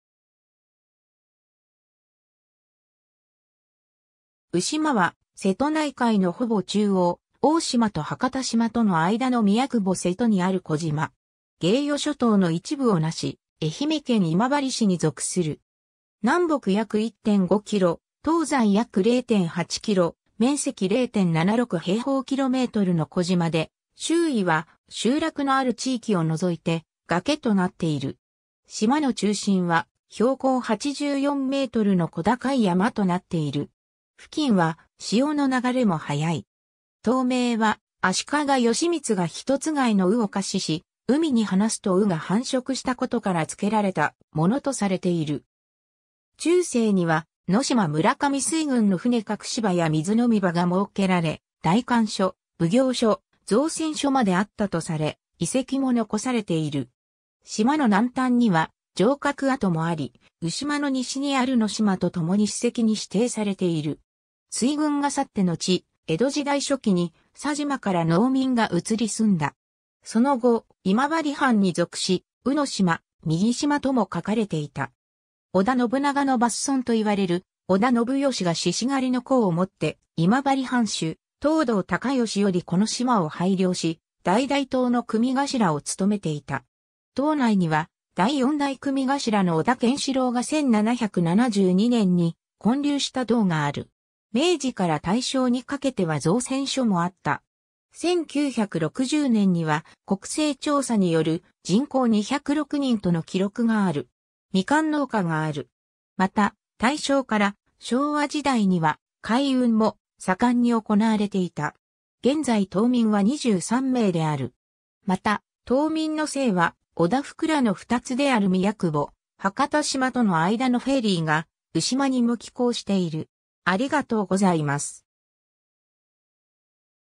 鵜島 (今治市)
鵜島（うしま）は、瀬戸内海のほぼ中央、大島と伯方島との間の宮窪瀬戸にある小島。
芸予諸島の一部をなし、愛媛県今治市に属する。
 南北約1.5キロ、東西約0.8キロ、面積0.76km2の小島で、周囲は集落のある地域を除いて崖となっている。
島の中心は標高84mの小高い山となっている。
付近は潮の流れも速い。
 島名は、足利義満が一つがいの鵜を下賜し、海に放すと鵜が繁殖したことから付けられたものとされている。
 中世には能島村上水軍の船隠し場や水のみ場が設けられ、代官所、奉行所、造船所まであったとされ、遺跡も残されている。
島の南端には城郭跡もあり、鵜島の西にある能島とともに史跡に指定されている。
 水軍が去ってのち、江戸時代初期に佐島（弓削島の属島）から農民が移り住んだ。
その後、今治藩に属し、「宇島」「右島」とも書かれていた。
織田信長の末孫といわれる織田信義が獅子狩の功をもって今治藩主・藤堂高吉よりこの島を拝領し、代々島の組頭を務めていた。
島内には、第4代組頭の織田権四郎が1772年（明和9年）に建立した堂がある。
 明治から大正にかけては造船所もあった。
1960年（昭和35年）には国勢調査による人口206人との記録がある。
みかん農家がある。
また，大正から昭和時代には海運も盛んに行われていた。
現在島民は23名である（平成27年国勢調査）。
また、島民の姓は、織田（平氏）福羅（源氏）の二つである 宮窪（大島）、伯方島との間のフェリーが鵜島にも寄航している。